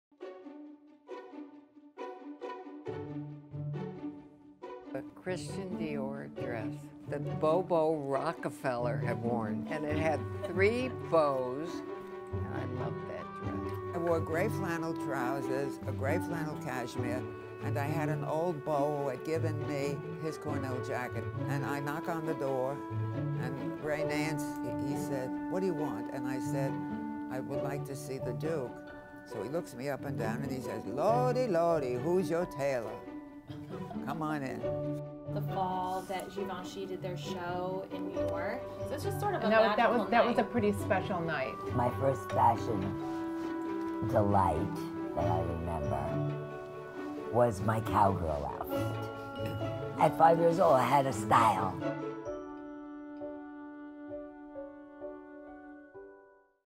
Drinks At BG: Just Us Girls | Bergdorf Goodman Magazine | Ep. 26
Press play as Linda Fargo and her most fashionable friends including Iris Apfel, Patricia Field, Ranjana Khan, Carole Radziwill and Bergdorf Goodman's own Betty Halbreich and Chloe King catch up over pink champagne and petit-fours at BG Restaurant. The topic of discussion? Their most memorable fashion moments of course.

Video by Ernest Martin

This Bergdorf Goodman video is about Fashion Director Linda Fargo and some of the women she admires most discussing their memorable fashion moments over drinks and petit-fours at BG Restaurant. Visit Linda's At Bergdorf Goodman, now on the fourth floor.